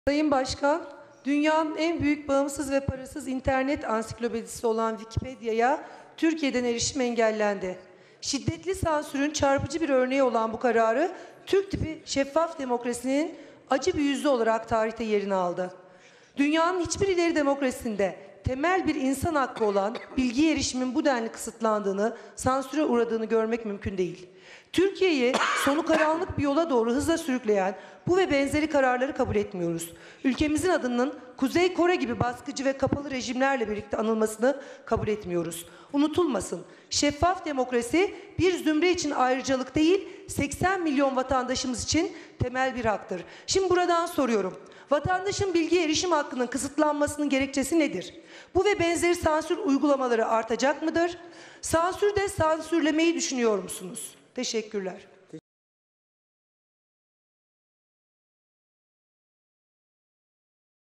Sansürü De Sansürlemeyi Düşünüyor Musunuz?
İstanbul Milletvekilimiz Gamze Akkuş İlgezdi: Dünyanın hiçbir ileri demokrasisinde, temel bir insan hakkı olan bilgiye erişimin bu denli kısıtlandığını, sansüre uğradığını görmek mümkün değil. Ülkemizin adının Kuzey Kore gibi baskıcı ve kapalı rejimlerle birlikte anılmasını kabul etmiyoruz.